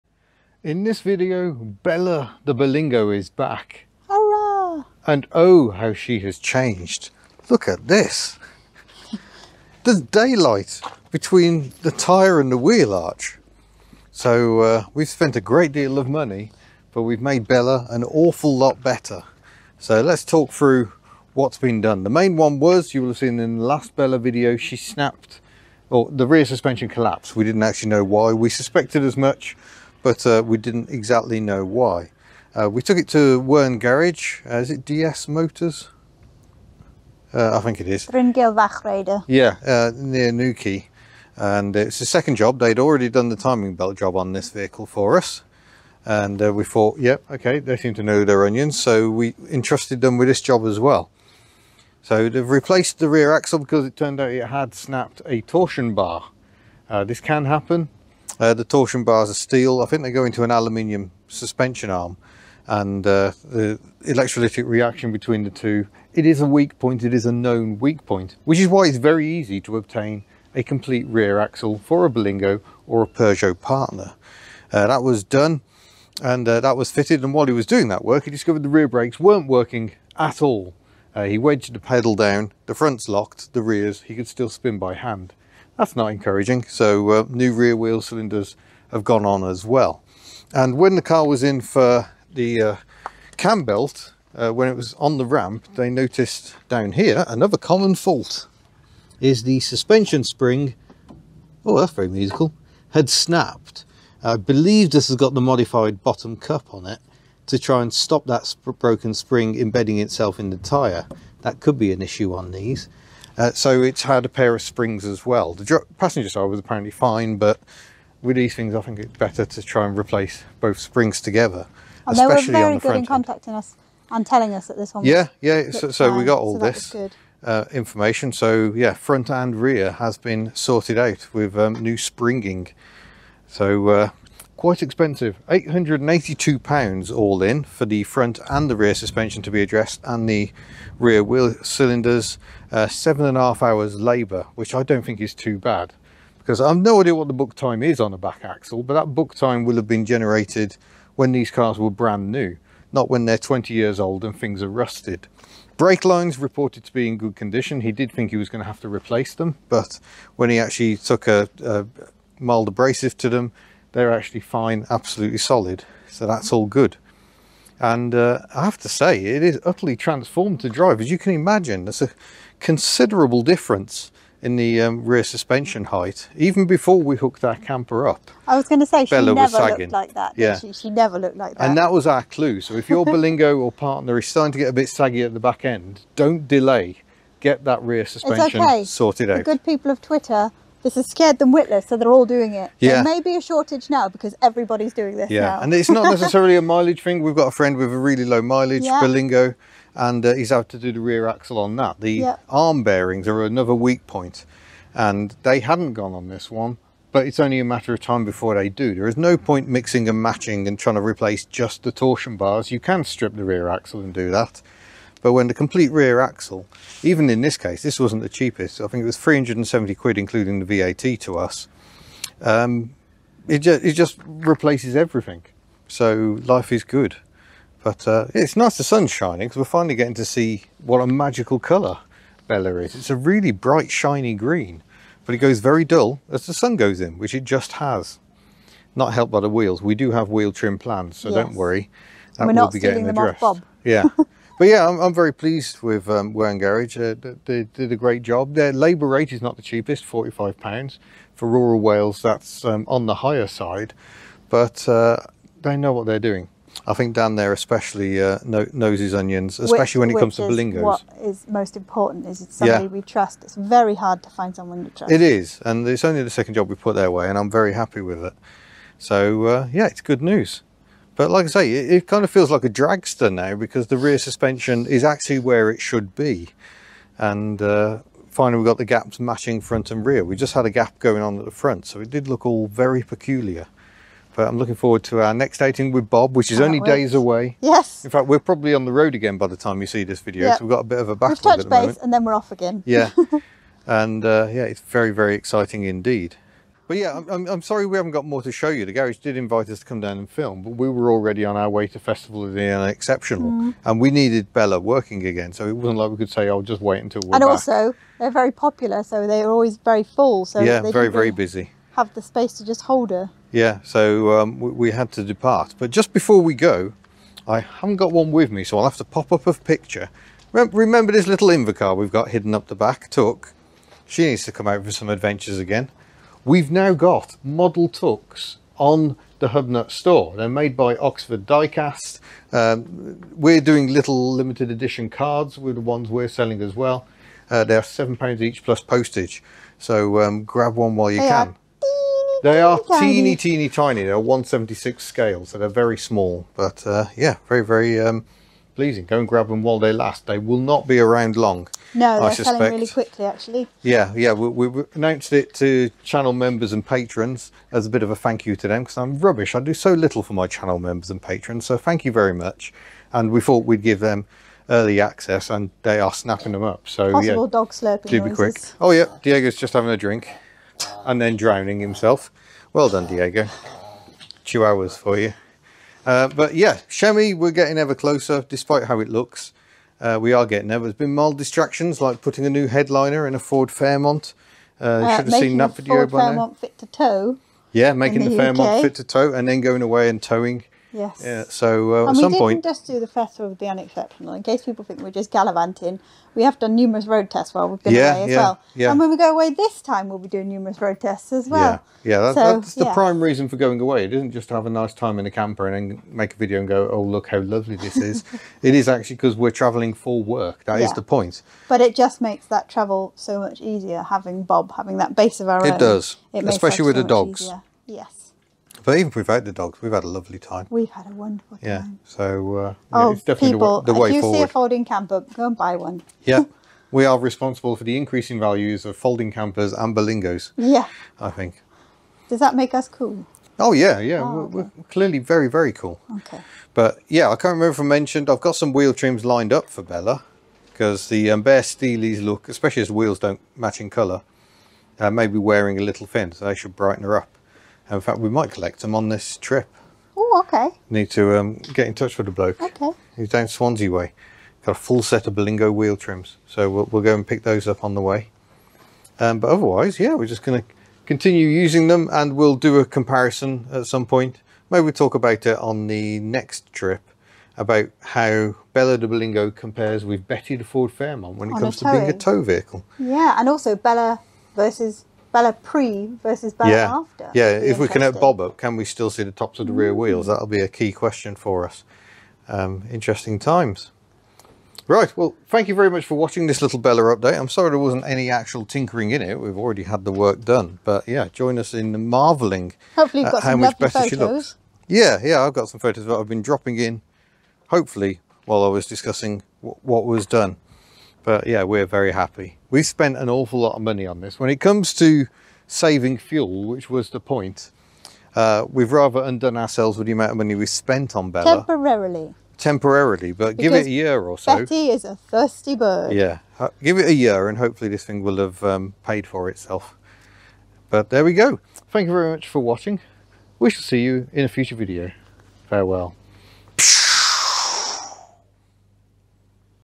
How much did it cost? Berlingo Rear suspension is fixed! (and the front...)
Sadly, we couldn't film the work, but here we discuss how Bella the Berlingo was re-suspended - front and rear! As we needed Bella back in action ASAP, the work was farmed out to a local garage. How much did it cost and how much difference did it make? The work done was rear axle replacement, new rear wheel cylinders and a pair of new front springs.

or at our Hoodie store